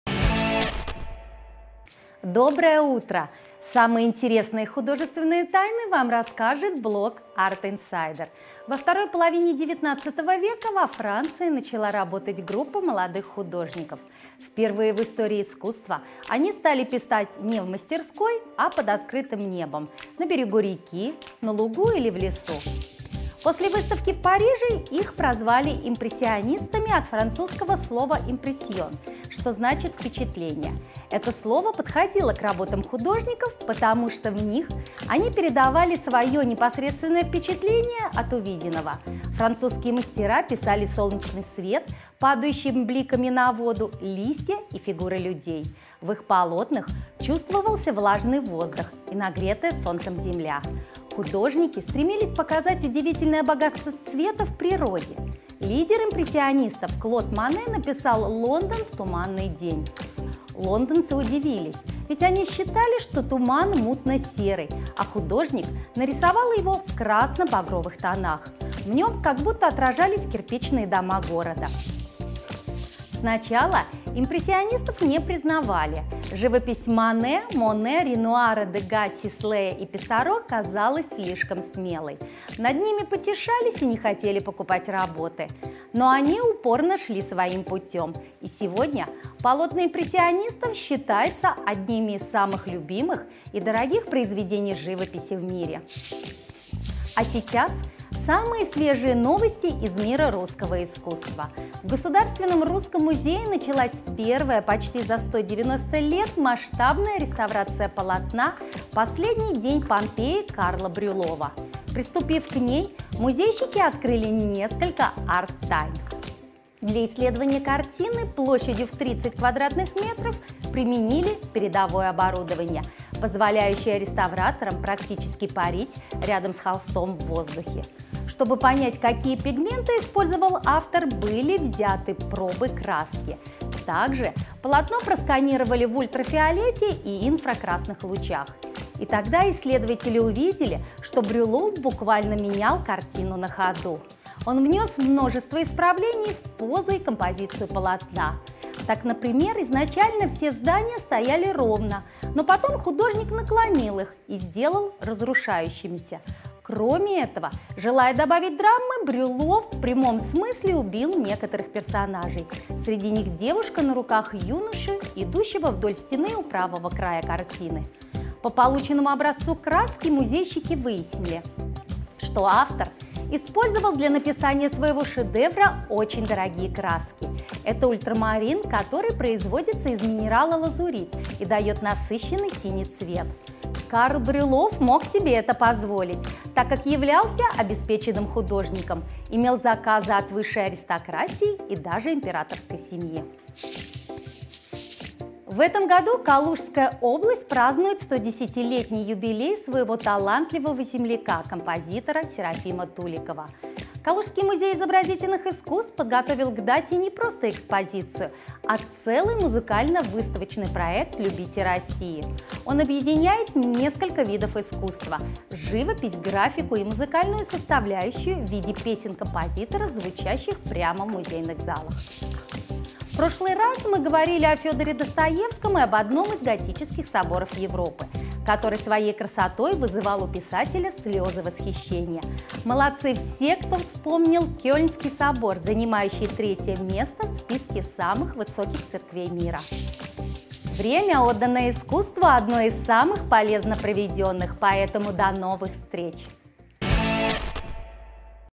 Импрессионисты | АРТ-ИНСАЙДЕР
О самом известном течении в искусстве конца 19 — начала 20 века и сенсационных тайнах полотна Брюллова узнаем от доверенного инсайдера Юлии Заложных. Не пропустите!